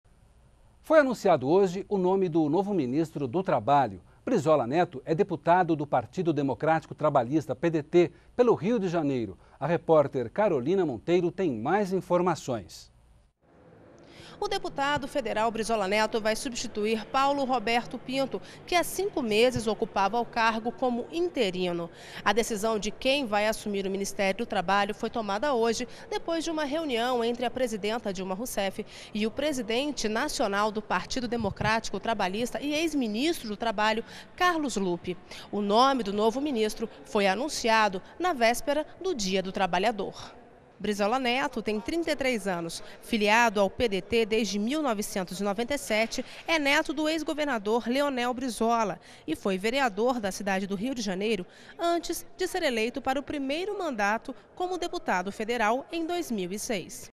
Brizola Neto é o mais jovem ministro do governo Dilma
NBR NOTÍCIAS - 30.04.12: Foi anunciado nesta segunda-feira (30) o nome do novo ministro do Trabalho. Aos 33 anos, Brizola Neto é o mais jovem ministro do governo da presidenta Dilma Rousseff. Neto do ex-governador Leonel Brizola, nasceu em Porto Alegre e é deputado do Partido Democrático Trabalhista (PDT/RJ).